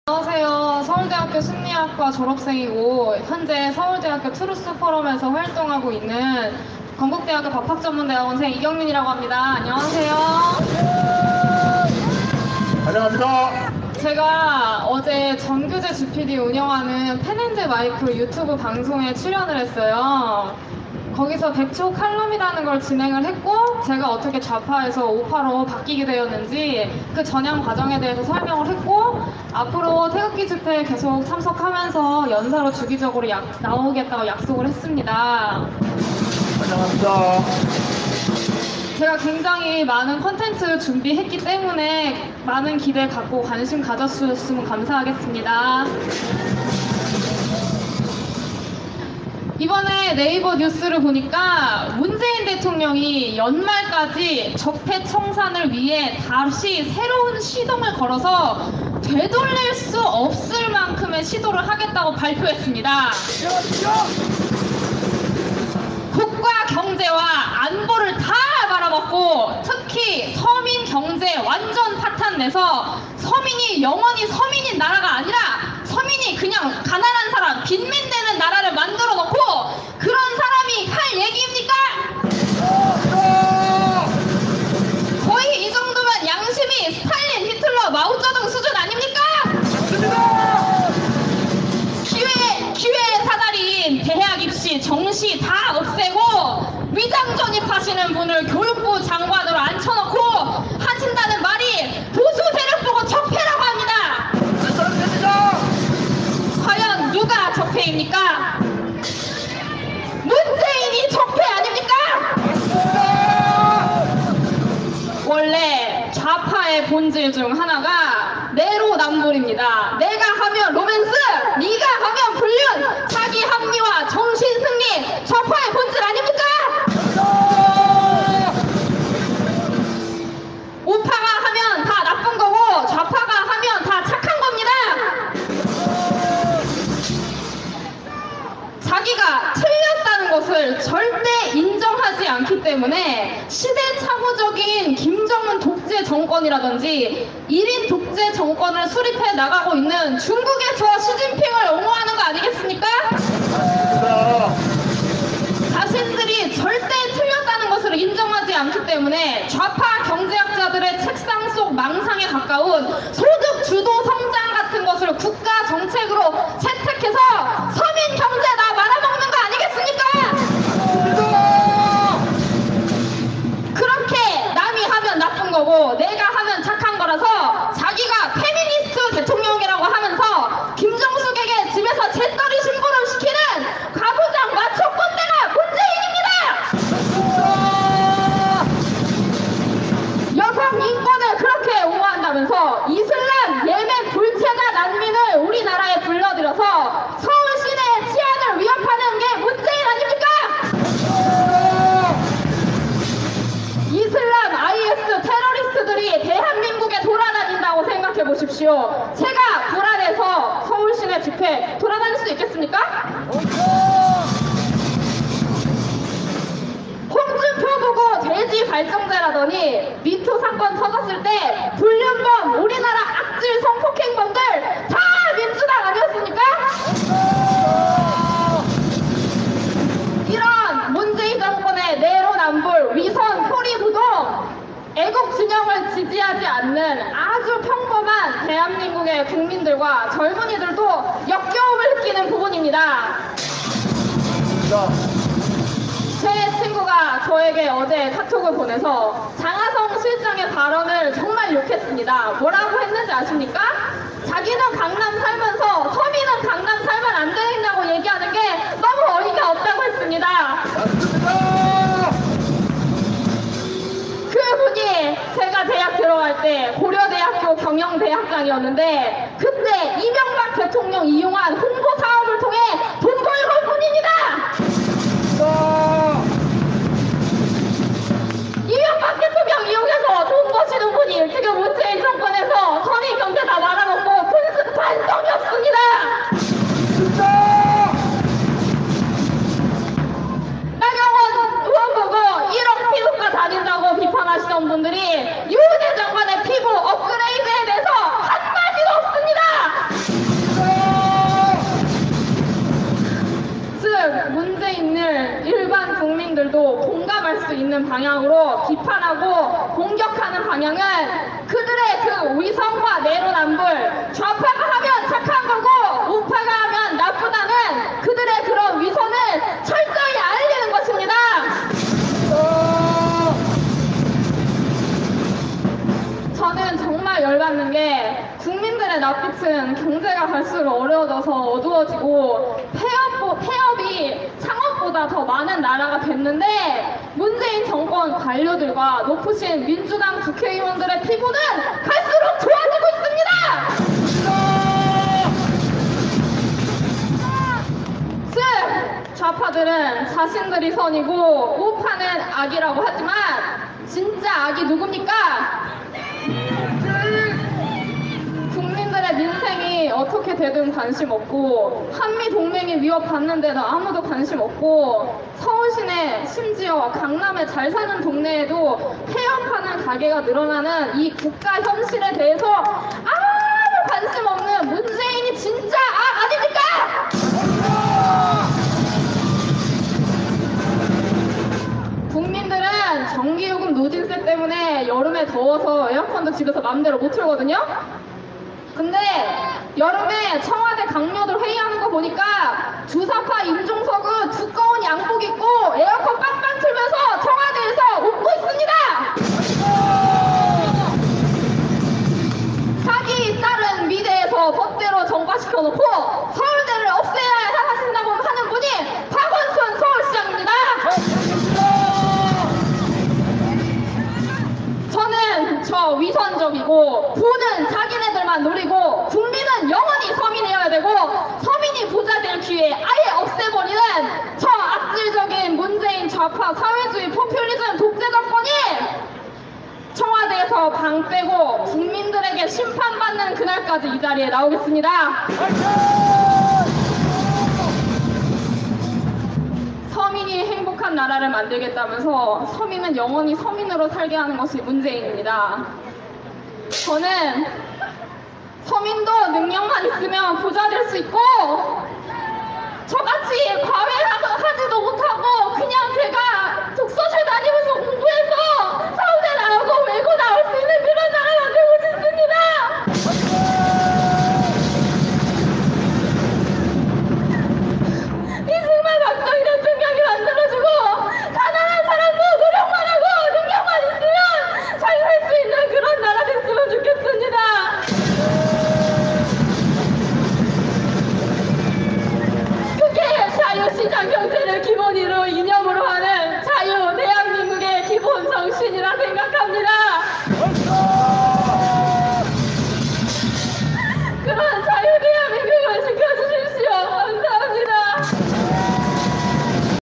법학도의 눈물-정의가 불의된 대한민국에 목놓아 우노라~
안녕하세요
서울대학교 심리학과 졸업생이고
현재 서울대학교 트루스 포럼에서 활동하고 있는 건국대학교 법학전문대학원생 이경민이라고 합니다.
어제 제가 정규재 주필이 운영하는 펜앤드 마이크 유투브 방송에 출연했습니다. 거기서 100초 칼럼이라는 것을 진행했고, 제가 어떻게 좌파에서 우파로 바뀌게 됐는지 설명했고, 앞으로 태극기집회에 주기적으로 참석 하면서 연사로 주기적으로 나오겠다고 약속했습니다.
제가 굉장히 많은 컨텐츠 준비 했기 때문에 많은 관심 가져 주셨으면 좋겠습니다

이번에 네이버 뉴스를 보니까 문재인 대통령이 이번 연말까지 적폐 청산을 위해 다시 새로운 시동을 걸어서 되돌릴 수 없을 만큼의 시도를 하겠다고 발표했습니다.
국가 경제와 안보를 다 말아먹고, 특히 서민 경제 완전 파탄 내서 서민이 영원히 서민인 나라가 아니라 서민이 그냥 빈민 되는 나라를 만들어 놓고
그런 사람이 할 얘기입니까!!!
거의 이정도면 거의 양심이 스탈린 히틀러 마오쩌둥 수준 아닙니까?

기회의 사다리인 대학입시 정시 다 없애고 위장전입 하시는 분을 교육부 장관으로 앉혀 놓고 하신다는 말이 보수세력보고 적폐라고 합니까!
과연 누가 적폐입니까?
문재인이 적폐 아닙니까?

원래 좌파의 본질 중의 하나가 내로남불입니다.
내가 하면 로맨스! 니가 하면 불륜!
자기합리화, 정신 승리. 좌파의 본질 아닙니까?
우파가 하면 다 나쁜 거고, 좌파가 하면 다 착한 겁니다!

자기가 틀렸다는 걸 절대 인정하지 않기에 아직도 시대착오적으로 김정은 독재 정권이라던지, 1인 독재 정권을 수립해 나가고 있는 중국의 저 시진핑을 옹호하는 것 아니겠습니까?
자기가 틀렸다는 걸 절대 인정하지 않기에! 좌파 경제학자들의 정신분열적 망상인 소득주도성장 같은 헛소리를 국가 정책에 반영해서 서민 경제 다 말아먹는 것 아닙니까?

그렇게 남이 하면 나쁜 거고, 내가 하면 착한거니까!
자기가 페미니스트 대통령이라면서 집에서 막상 김정숙한테 재떨이 심부름 시키는 가부장 마초 꼰대가 문재인입니다!

여성 인권을 그렇게 옹호한다면서 이슬람 예멘 불체자 난민을 우리나라에 불러들여서 서울시내 치안을 위협하는게 문재인 아닙니까!! 이슬람 IS 테러리스트들이 우리나라에 돌아다닌다고 생각해 보십시오. 제가 불안해서 서울시내 집회, 돌아다닐 수 있겠습니까?
홍준표보고 돼지 발정제라더니, 미투 사건 터졌을 때 불륜범, 우리나라 악질 성폭행범들
다~ 민주당 아니었습니까?

이런 문재인 정권의 위선, 표리부동, 내로남불
애국진영을 지지하지 않는 아주 평범한 대한민국의 국민들과 젊은이들도 역겨움을 느끼는 부분입니다.

제 친구가 어제 저한테 카톡을 보내서 장하성 실장의 발언을 정말 욕했습니다. 뭐라고 했는지 아십니까? 자기는 강남 살면서 서민은 강남 살면 안된다고 하는게 너무 어이없다고 했습니다.
그 분이 제가 대학 들어갈 때 고려대 경영대학장이었는데, 그 때 이명박 대통령 이용한 홍보사업을 통해 돈 벌던 분입니다!! 이명박 대통령 이용해서 돈 버시는 분이 문재인 정권에서 서민경제 다 말아먹고도 도무지 반성이 없습니다!!

나경원 의원보고 1억 피부과 다닌다고 비판하시던 분들이 유은혜 장관의 피부
업!그!레!이!드에 대해선 한마디도 없습니다!!

즉!! 문재인을 일반 국민들도 공감할 수 있는 방향으로 비판하고 공격하는 방향은
그들의 그 위선과 내로남불, 좌파가 하면 착한거고 우파가 하면 나쁘다는 그들의 그런 위선을 철저히 알리는 것입니다!!

저는 정말 열 받는게.
국민들의 낯빛은 경제가 갈수로 어려워져서 어두워지고 폐업이 창업보다 많은 나라가 됐는데, 문재인 정권 각료들과 민주당 높으신 국회의원들의 피부는 갈수록 좋아지고 있습니다!

즉!!
좌파들은 자기들이 선이고 우파는 악이라 하지만
진짜 악이 누굽니까!

국민들의 민생이 어떻게 되든 관심 없고
한미 동맹이 위협받아도 관심 없고
서울시내 심지어 강남의 잘 사는 동네에도 페업하는 가게가 늘어나는데도
이 국가 현실에 대해서 아무~ 관심 없는 문재인이 진짜 악 아닙니까!!

국민들은 전기요금 누진세 때문에 여름에 더워서 에어컨도 집에서 맘대로 못틀거든요?
근데 여름에 청와대 각료들 회의하는거 보니까 주사파 임종석은 두꺼운 양복 입고 에어컨 빵빵 틀면서 청와대에서 웃고 있습니다!

자기 딸은 미대에서 법대로 전과시켜 놓고 서울대를 없애야 한다는 분이 박원순 서울시장입니다!

저는 저 위선적이고, 부는 자기네들만 누리고, 국민은 영원히 서민이어야 되고 서민이 부자될 기회 아예 없애버리는 저 악질적인  문재인 좌파 사회주의 정권이 청와대에서 방 빼고
국민들에게 심판받는 그날까지 이 자리에 나오겠습니다!

서민이 행복한 나라를 만들겠다면서 서민은 영원히 서민으로 살게 하는 것이 문재인입니다.
저는 서민도 능력만 있으면 부자 될 수 있고!
저같이 과외 안하고 그냥 제가 독서실 다니면서 공부해서 서울대 나오고 외고 나올 수 있는 그런 나라 만들고 싶습니다….

이승만 박정희 대통령이 만들어주고 가난한 사람도 노력하고 능력만 있으면 잘 살 수 있는 그런 나라 됐으면 좋겠습니다.
그게 자유시장경제를 기본 이념으로 하는 자유대한민국의 기본정신이라 생각합니다.
그런 자유대한민국을 지켜주십시오.
연설문

여러분들 안녕들하십니까?
 노인들만 있을줄 알았던 이 광화문집회에 저 같은 청년이 왜 나왔나 싶죠?
 티비에서는 박사모라 그러고, 돈받고 나왔다 그러는데 신기하죠?
 저 박사모도 아니고, 돈받고 나오지도 않았습니다.

 대통령 바뀐지 1년 4개월지났습니다.
 대통령 바뀌고  여러분 삶이 조금 나아졌습니까?

 촛불혁명, 촛불혁명 그러는데 도대체 무슨 혁명이 일어났습니까?
 두대통령이 희대의 악마로 묘사되며 감옥으로 갔습니다
 아직도 쥐새끼, 닭새끼 뭐가 그렇게 미울까요?
 그 미운 두 대통령 감옥가니깐 여러분 삶이 좀 나아졌습니까?
 아버지 장사 잘된답니까?, 여러분들 취업 잘된답니까?
치솟는 집값에 도대체 내집 마련은 언제합니까?
촛불과 국민을 팔아대던 그들 주머니는 두둑해지는데
왜 내 주머니는 자꾸 궁해질까요?

 문재인 대통령이 옜날부터 4대강 4대강 하는데
 여러분들 4대강 사업해서 여러분들 삶이 힘들어졌습니까?
한번 잘 생각해보십시요, 그냥 티비뉴스에서 하도 나쁘다 나쁘다하니깐
그런거 아닌지.
 정말 한번 생각해보시기 바랍니다. ㅠ

 이승만, 박정희 대통령에 대해 짧게 얘기할까 합니다.
 요즘 이 두 전직 대통령을 뭐라고 합니까? 독재자 독재자 그럽니다.
 또 뭐라고 합니까? 친일파 친일파 라고 합니다.

 근데 여러분들 상식적으로 한번 생각해보십시오
 이승만 박정희 시대때 우리는 북한보다도 못사는 나라였습니다.
 근데 친일파 이고 독재자인데 어떻게 국민들은 더 잘살게 됐을까요?
 친일파면 일본한테 돈도 갖다 바치고, 나라고 팔고 해야하는데
 왜 우리는 일본만큼이나 잘 살게 됐을까요?

 이명박 대통령때는 일본의 많은 경제학자들이 대한민국을 많이
부러워 할정도 입니다.
 아니 친일파인데 왜 일본이 부러워합니까?

 근데 지금은 어떻습니까?
 대한민국 경제는 최악인데 일본, 미국은 최고의 호황을 누리고 있습니다.
 북한한테 석탄사주고, 쌀 갖다주고 아니 도대체 우리가 빚진게 뭐가 있길래
 북한한테 그럽니까?
 진정 북한 공산당 정권을 이롭게 하고, 옆나라 일본에게 국가경쟁력은 자꾸 밀리고 있는데 이런 문재인 정부가 친북파이고 친일파아닙니까?
 여러분들은 잘못된 사실에 속았습니다.
 이제는 정말 속지말고 깨어 나십시오!